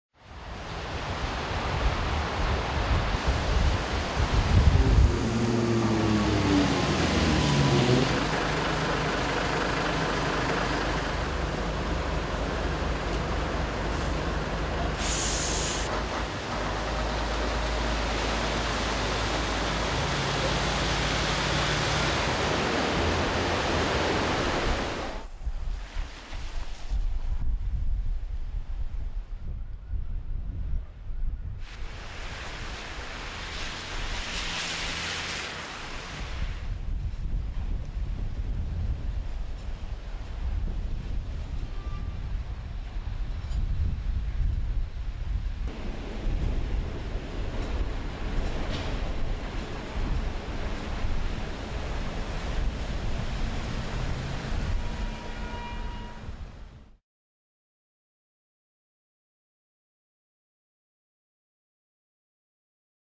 الطقس في لبنان: مقتل شخصين بسبب الفيضانات جنوب بيروت
قتل شخصان على الأقل في العاصمة اللبنانية بيروت بسبب هطول غزير للأمطار الإثنين. وقد تسببت الأمطار في فيضانات، وأدت الى تدمير للمرافق والمنازل واقتلاع الأشجار.